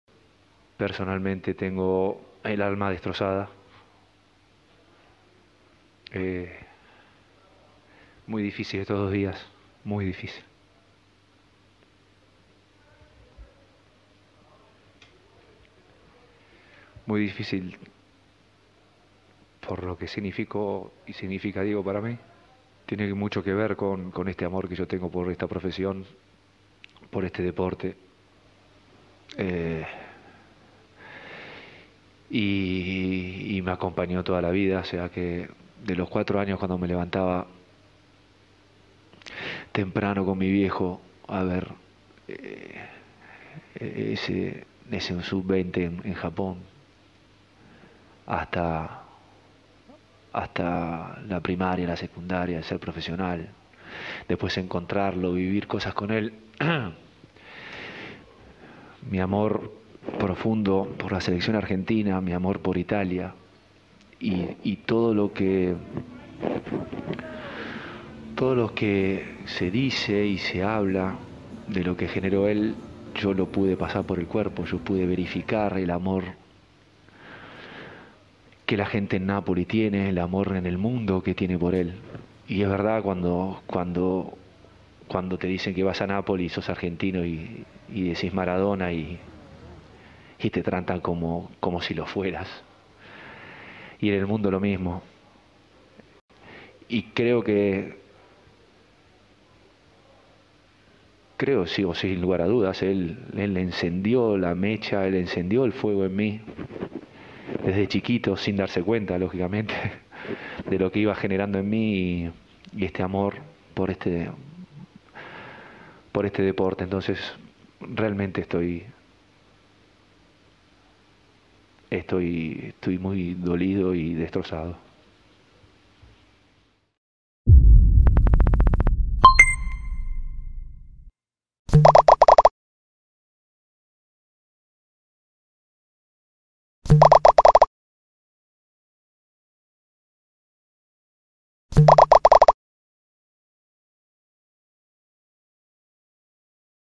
El dolor de Crespo por Maradona: "Tengo el alma destrozada" I MARCA
El argentino recordó a su compatriota y revivió recuerdos de su carrera deportiva y personal.